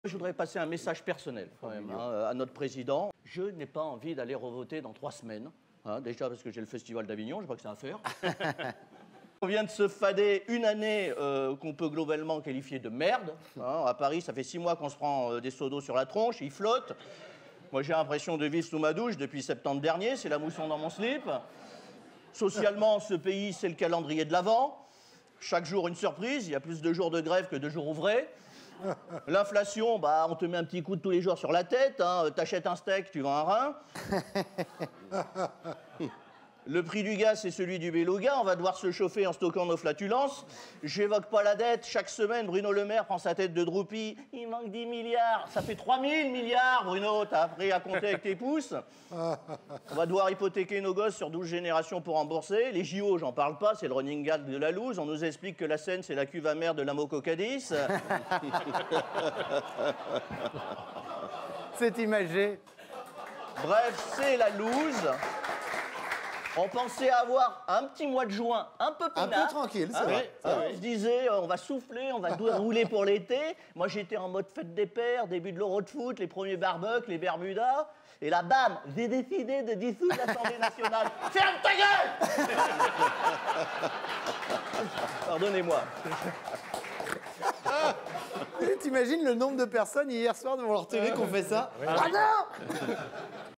90 secondes de raz le bol citoyen
Dette, pluie, inflation, J.O, pouvoir d'achat en berne Régis Mailhot en a marre...
Je n'ai pas envie de revoter !  Attention à la chute !
"on nous explique que la seine,  c'est la vue à merde de l'amoco-cadiz"
REGIS MAILHOT SUR SCENE A AVIGNON